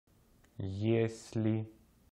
Russian word "если" – Russian Pronunciation for Beginners
Russian word "если" pronounced by a native speaker.

IPA phonetic transcription: jˈeslʲɪ
Transcription with Cyrillic letters: jе́сл'и(е)
English translation: if
French translation: si